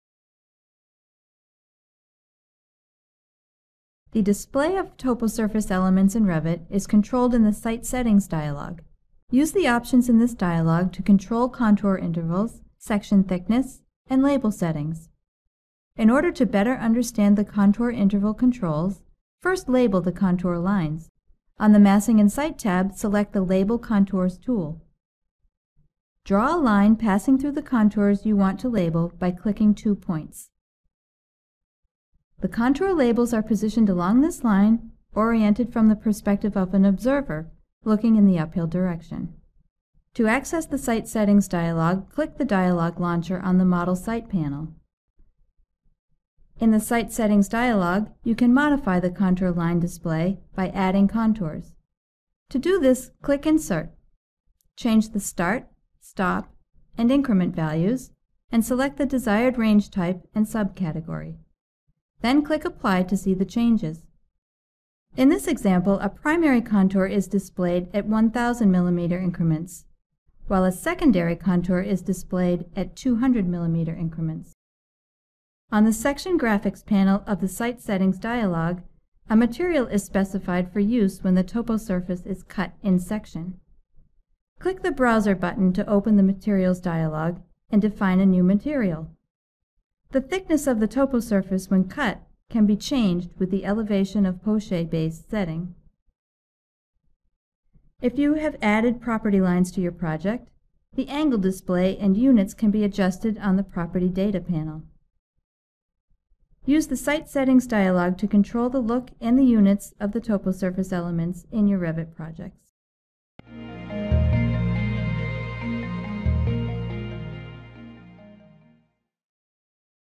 Autodesk Revit Architecture: Site Settings
Demonstrates how to access the Site Settings dialog in Revit Architecture in order to label and control the display of contours and the sectional qualities of the toposurface.